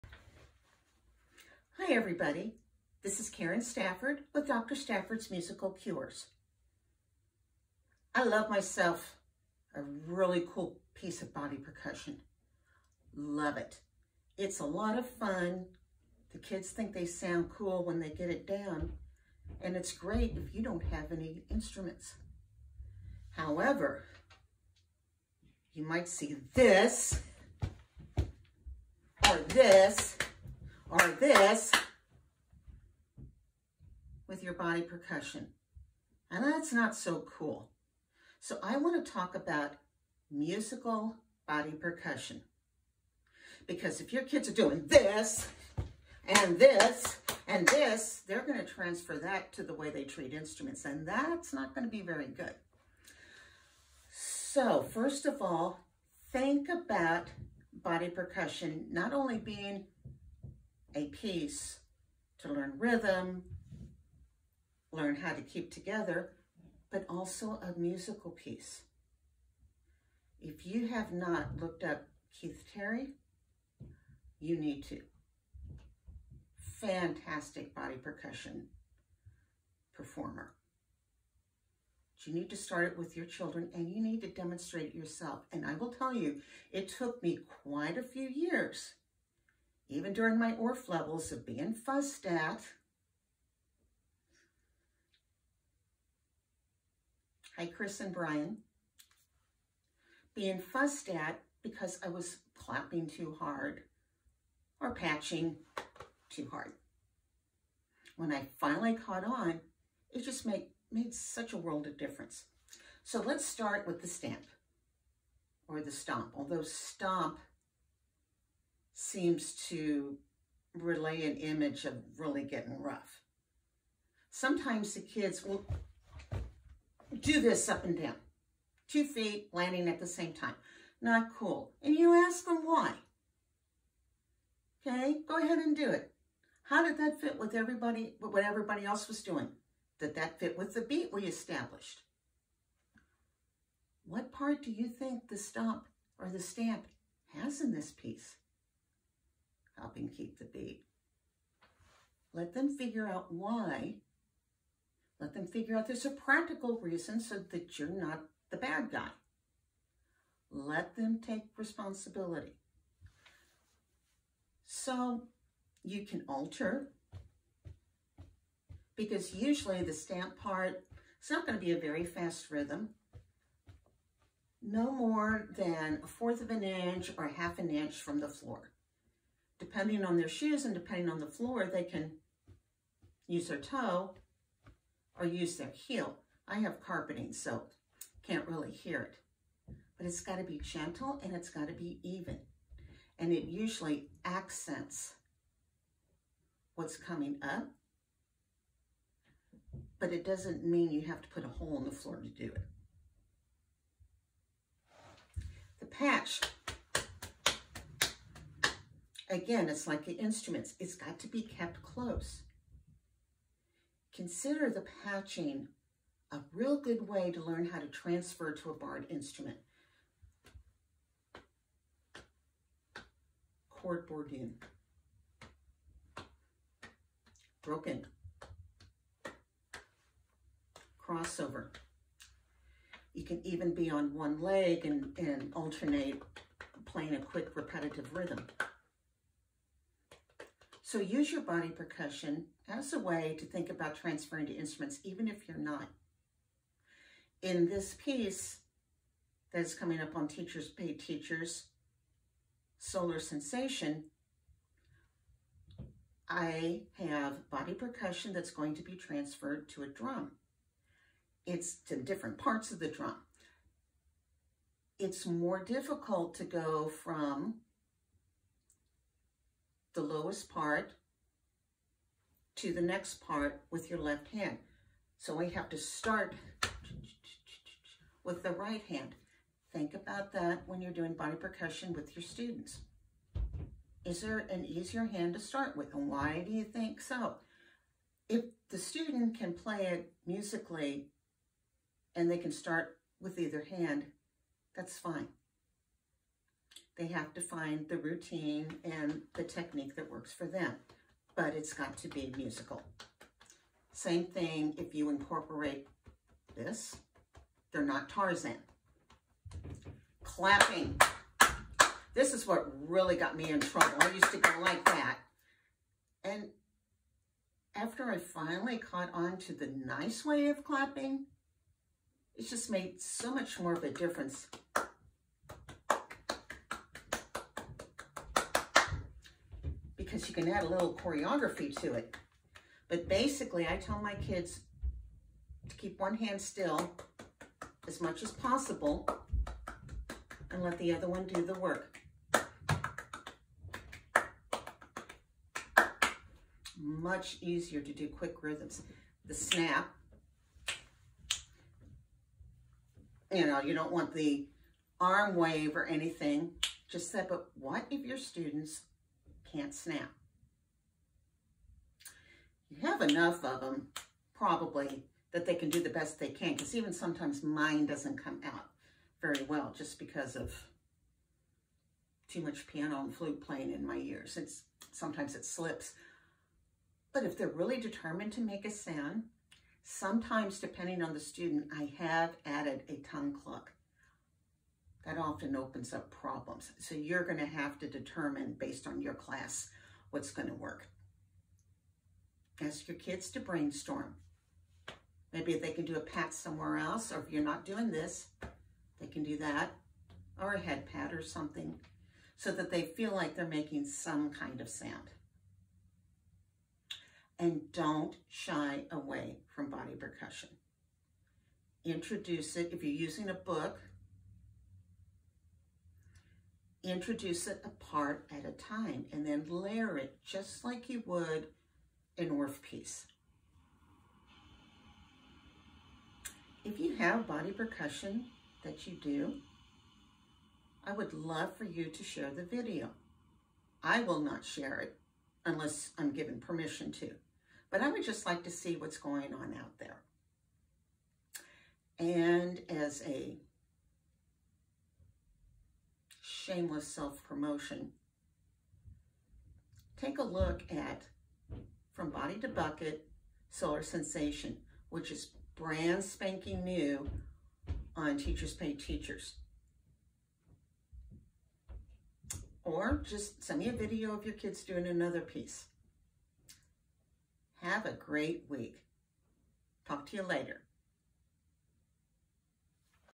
Musical Body Percussion
This video demonstrates the improper way children might attempt body percussion, and the musical way you can teach your students.